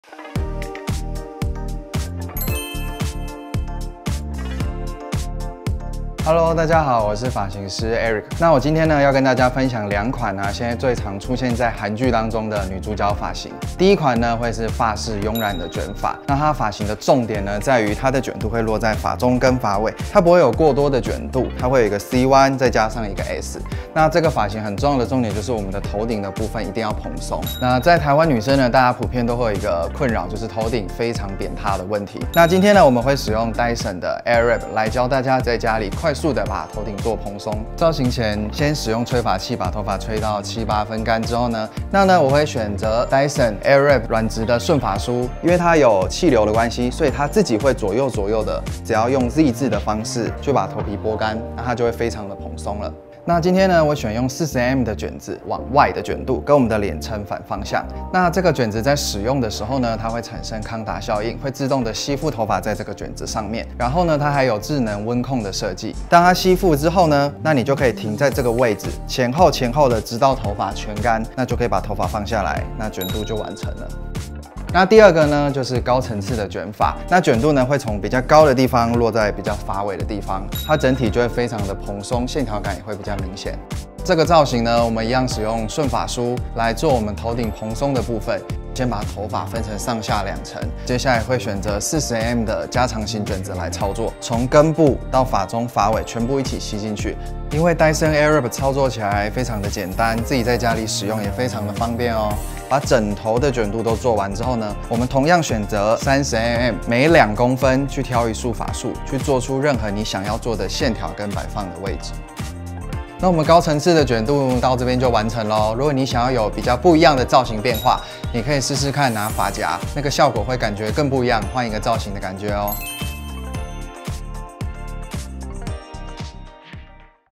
人氣韓星必備「慵懶蓬鬆系捲髮」！2021春夏熱門髮型範本推薦
有在關注韓國女團或是韓劇的女生們應該不難發現，近年韓國女星大勢的捲髮都有著一個相同的特點，就是走「慵懶蓬鬆感」！這類的捲髮造型不僅給人不經意、率性的氛圍，又能凸顯女性優雅與性感。不過到底這種慵懶蓬鬆系捲髮要怎麼造型呢？這次VOGUE特別邀請藝人御用的知名髮型師Eric，來為大家解說與示範重點技巧。